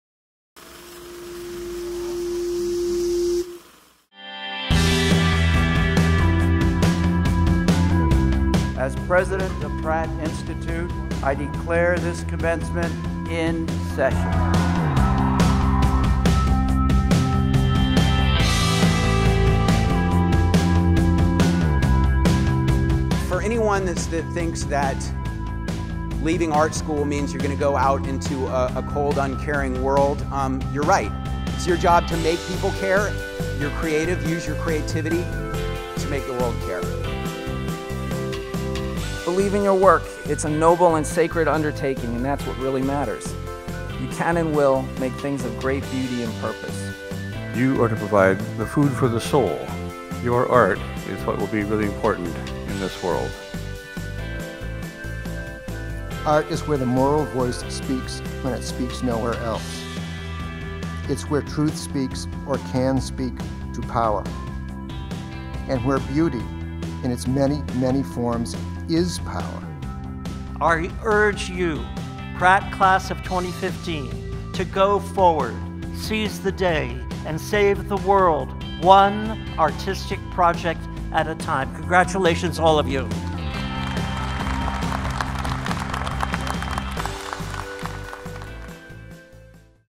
Commencement 2015
Pratt Institute held its 126th Commencement ceremonies on May 15, 2015 at New York City's Theater at Madison Square Garden, where 1,255 graduate and undergraduate students crossed the stage to receive their degrees and become alumni.